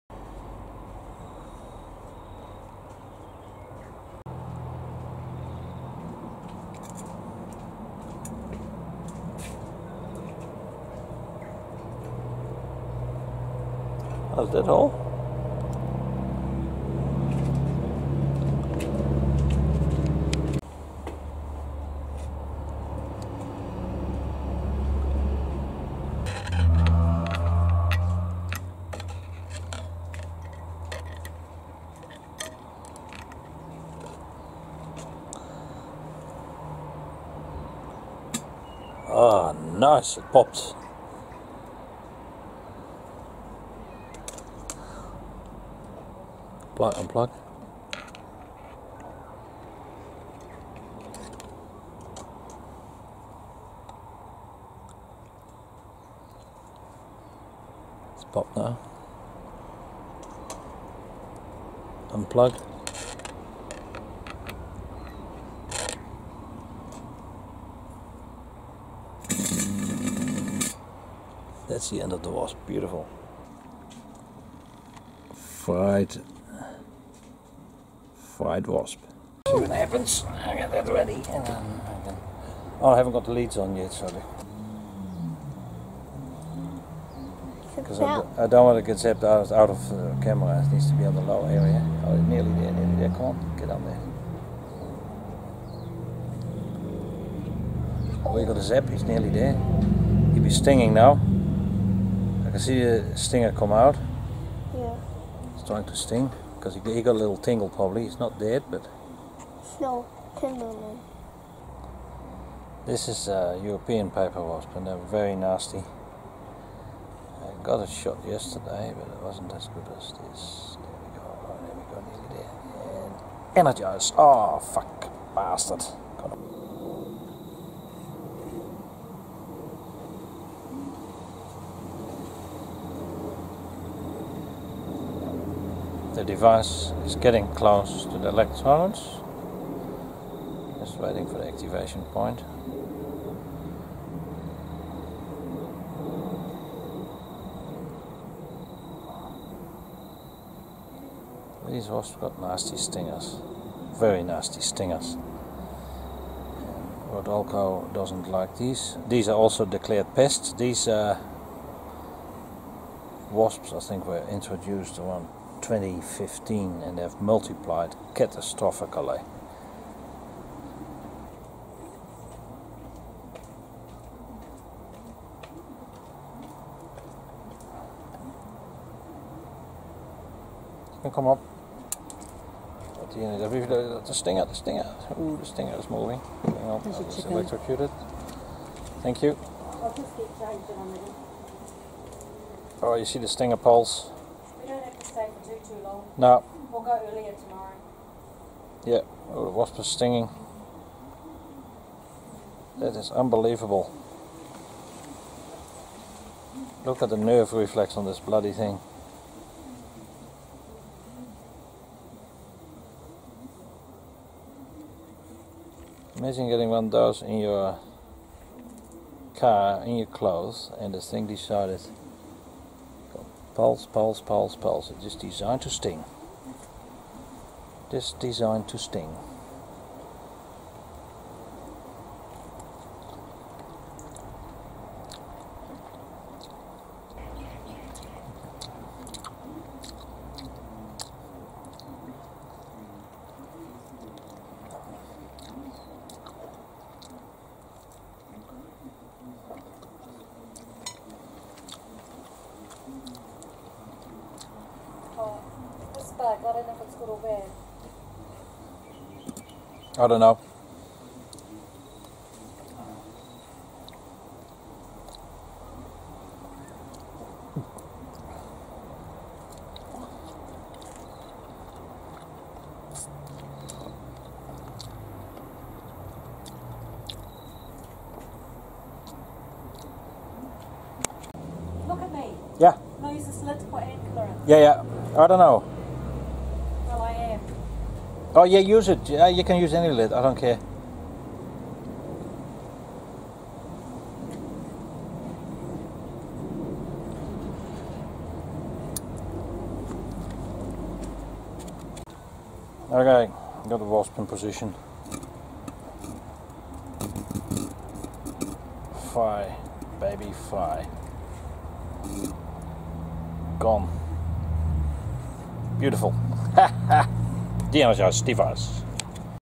Watch those stingers
European or Tasmanian paper wasp.
Pretty flexible body.
Nasty stinger
You see the wasp squirt vermin at 0:08

These wasps are a declared pest in New Zealand.